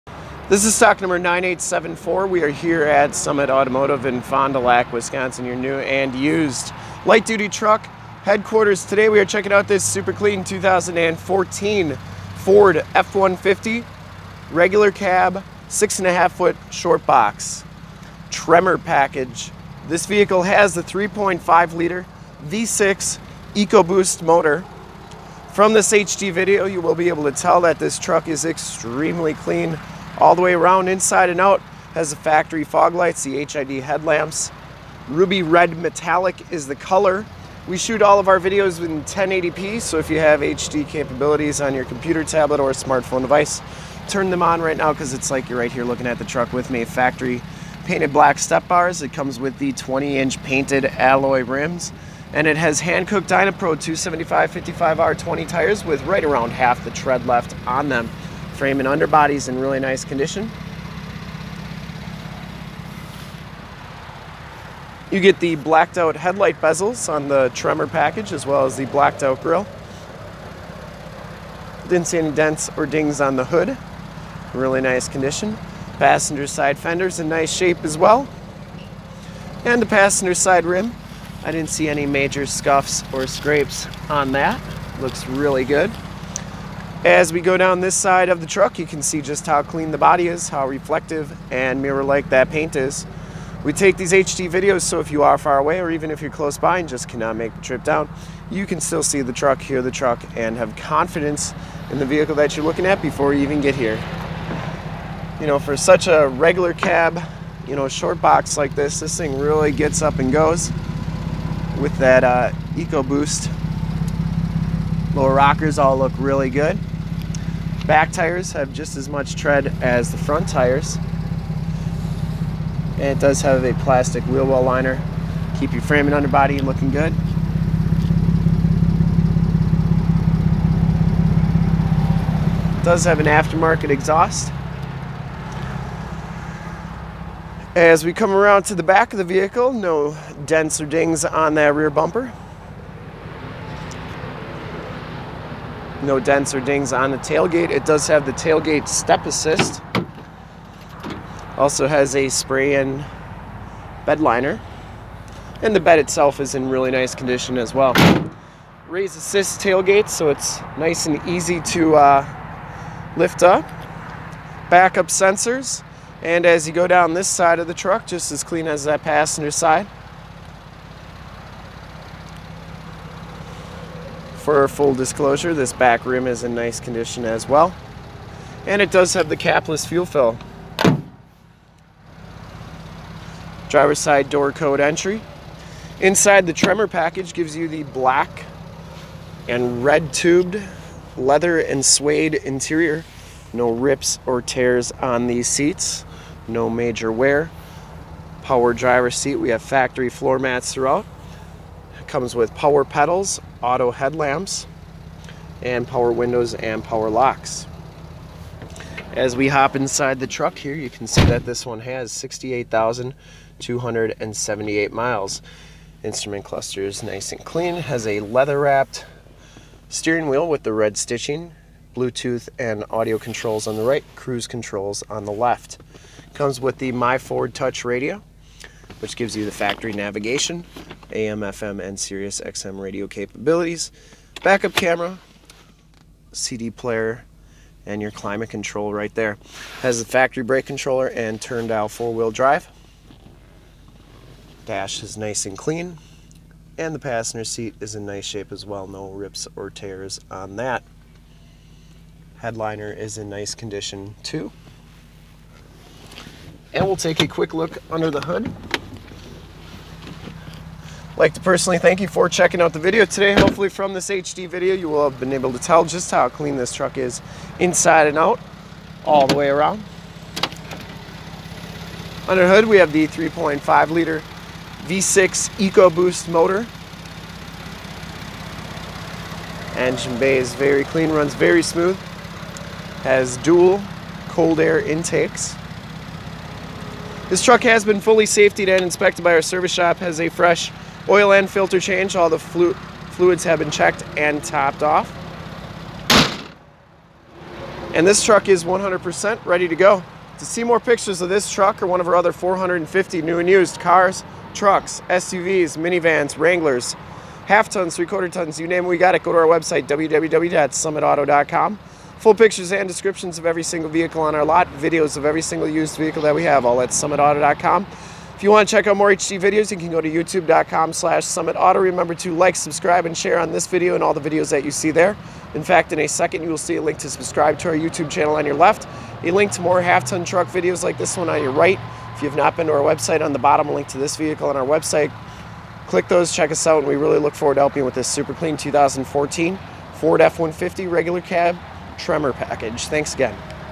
2014 FORD F150 TREMOR PACKAGE REGULAR CAB WALK AROUND REVIEW SOLD! 9874
This 2014 Ford F150 Tremor edition with the 3.5 Liter Ecoboost in Ruby Red Metallic is the truck we did a video walk around review of today.

Thank you for checking out rhis video of this 2014 Ford F150 Regular Cab Tremor Edition Truck.

STOCK: 9874
PRICE: $24,999
MILES: 68,249
MAKE: FORD
MODEL: F150
VIN: 1FTMF1ETXEFB09952
LOCATION: FOND DU LAC OSHKOSH WISCONSIN, 54937 TRUCKS ON 41

CLEAN TITLE HISTORY! 3.5 Liter Turbocharged V6 Twin-turbo Ecoboost Engine, 365 Horsepower, Tremor Plus Package, Two Door Regular Cab, Short Box 6 1/2 Foot Shortbox, FX Appearance and FX4 Offroad Suspension Package, 6 Speed Automatic Transmission, Automatic Transmission Center Console Shifter, Turn Dial 4x4 Four Wheel Drive 4WD, Factory GPS Navigation System, Reverse Backup Camera Rearview Camera, Power Driver's Seat, Black Ebony Leather Seats, Suede and Leathers Seats, Bucket Seats, 4.10 Gears with Limited Slip Differential, 20 Inch Rims Premium Wheels, Painted Alloy Rims Premium Wheels, Four Wheel Disc Brakes, Factory Painted Stepbars, Spray-in Bedliner, Bedrail Covers, Fog Lights, HID High Intensity Discharge Headlights, Reverse Sensors, Securicode Driver Side Doorcode Keyless Entry, Easy Fuel Capless Fuel Fill, Locking Tailgate, Tailgate Step Assist Manstep, AM / FM Radio Tuner, Sirius/XM Satellite Radio Capabilities Sirius / XM, CD Player, MyFord Touch System, Microsoft SYNC System with Bluetooth, Hands-Free Audio System Blue Tooth, SD Card Slot, USB Jack Portable Audio Connection, Keyless Entry with Factory Remote Start, Rear Window Defroster, Adjustable Height Seatbelts, Driver and Passenger Front Air Bags, L.A.T.C.H. Child Safety System, Side Curtain Air Bags SRS Safety Restraint System, Multi-Function Steering Wheel Controls, Compass, Outside Temperature Display and Mileage Display, Power Adjustable Pedals, Factory Floormats, Air Conditioning AC, Cruise Control, Power Locks, Power Windows, Automatic Headlights Autolamp, Tilt/Telescope Steering Wheel, Ruby Red Metallic Tinted Clearcoat, CLEAN AUTOCHECK! This is one of the sharpest

STOCK: 9874
PRICE: $24,999
MILES: 68,249
MAKE: FORD
MODEL: F150
VIN: 1FTMF1ETXEFB09952